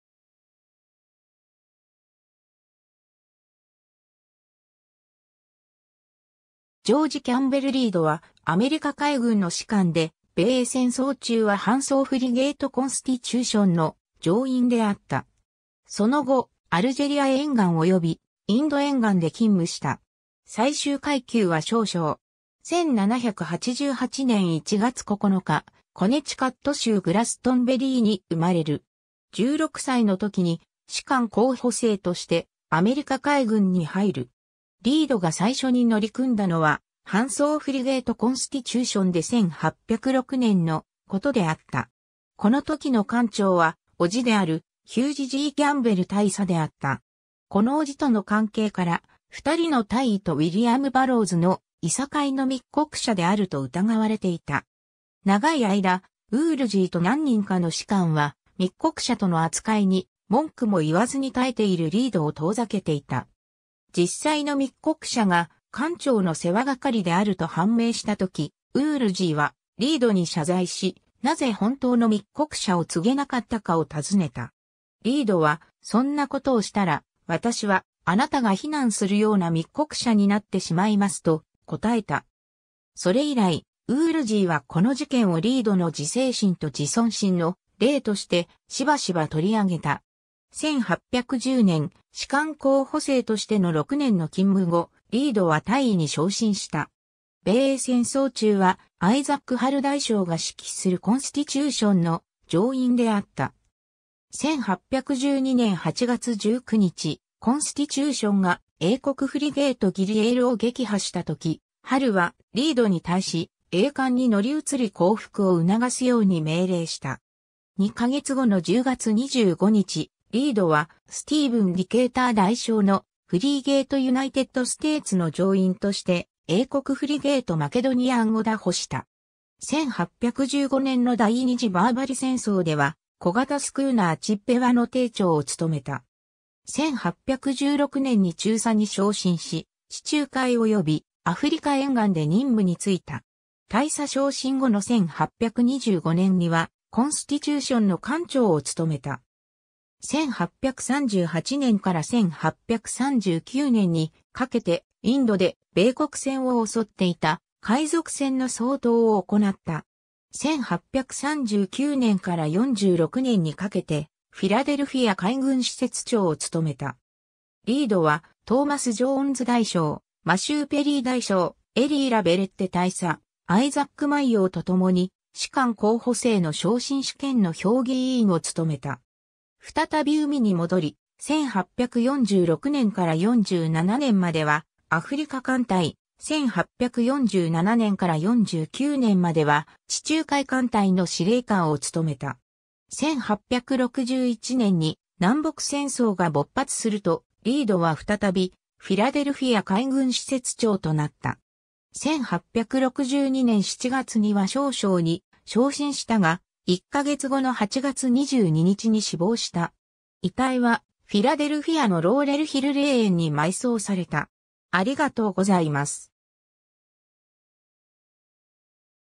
ジョージ・リード (アメリカ海軍士官)
ジョージ・キャンベル・リード（英語：George Campbell Read、1788年1月9日 – 1862年8月22日）はアメリカ海軍の士官で、米英戦争中は帆走フリゲート・コンスティチューションの乗員であった。
その後、アルジェリア沿岸およびインド沿岸で勤務した。
最終階級は少将。
 1788年1月9日、コネチカット州グラストンベリーに生まれる。
16歳の時に、士官候補生としてアメリカ海軍に入る。
リードが最初に乗組んだのは、帆走フリゲート・コンスティチューションで1806年のことであった。
このときの艦長は、叔父であるヒュージ・G・キャンベル大佐であった。
この叔父との関係から、二人の大尉（メランクトン・ウールジー（Melancthon Woolsey）とウィリアム・バロウズ（William Burrows））の諍いの密告者であると疑われていた。
長い間、ウールジーと何人かの士官は、密告者との扱いに文句も言わずに耐えているリードを遠ざけていた。
実際の密告者が艦長の世話係であると判明したとき、ウールジーはリードに謝罪し、なぜ本当の密告者を告げなかったかを尋ねた。
リードは「そんなことをしたら、私はあなたが非難するような密告者になってしまいます」と答えた。
それ以来、ウールジーはこの事件をリードの自制心と自尊心の例として、しばしば取り上げた。
 1810年、士官候補生としての6年の勤務後、リードは大尉に昇進した。
米英戦争中はアイザック・ハル代将が指揮するコンスティチューションの乗員であった。
1812年8月19日、コンスティチューションが英国フリゲート・ギリエール（HMS Guerriere）を撃破したとき、ハルはリードに対し、英艦に乗り移り降伏を促すように命令した。
2ヵ月後の10月25日、リードはスティーブン・ディケイター（Stephen Decatur ）代将のフリゲート・ユナイテッドステーツ（USS United States）の乗員として、英国フリゲート・マケドニアン（HMS Macedonian）を拿捕した。
 1815年の第二次バーバリ戦争では、小型スクーナー・チペワ（USS Chippewa）の艇長を務めた。
1816年に中佐に昇進し、地中海およびアフリカ沿岸で任務についた。
大佐昇進後の1825年にはコンスティチューションの艦長を務めた。
1838年から1839年にかけて、インドで米国船を襲っていた海賊船の掃討を行った（東インド艦隊司令官）。
 1839年から46年にかけて、フィラデルフィア海軍施設（Philadelphia Naval Asylum）長を務めた。
リードはトーマス・ジョーンズ（Thomas Jones）代将、マシュー・ペリー代将、エリー・ラ・ヴェレッテ（Elie La Vallette）大佐、アイザック・マイヨー（Isaac Mayo）と共に、士官候補生の昇進試験の評議委員を務めた。
 再び海に戻り、1846年から47年まではアフリカ艦隊（African Squadron）、1847年から49年までは地中海艦隊（Mediterranean Squadron）の司令官を務めた。
 1861年に南北戦争が勃発すると、リードは再びフィラデルフィア海軍施設長となった。
1862年7月には少将に昇進したが、1ヶ月後の8月22日に死亡した。
遺体はフィラデルフィアのローレルヒル霊園に埋葬された。